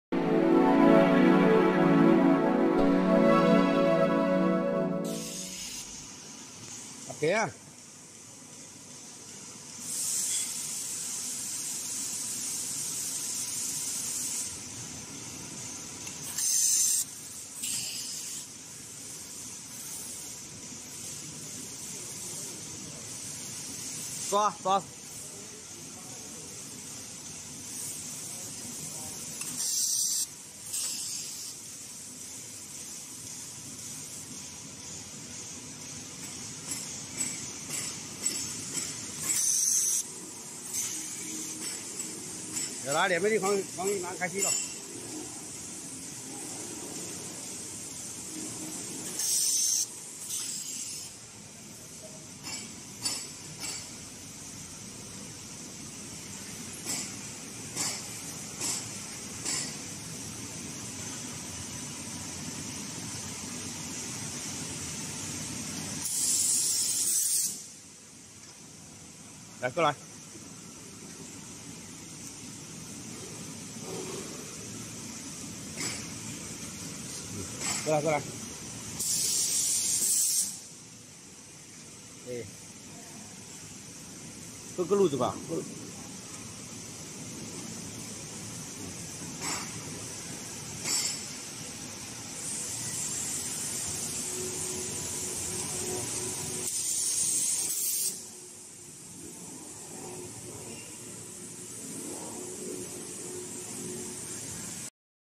Low price Facial tissue Napkin paper bagging sealing packing machine ZH-15 + FT-25
ZH-15 bagging machine + FT-25 both side sealing machine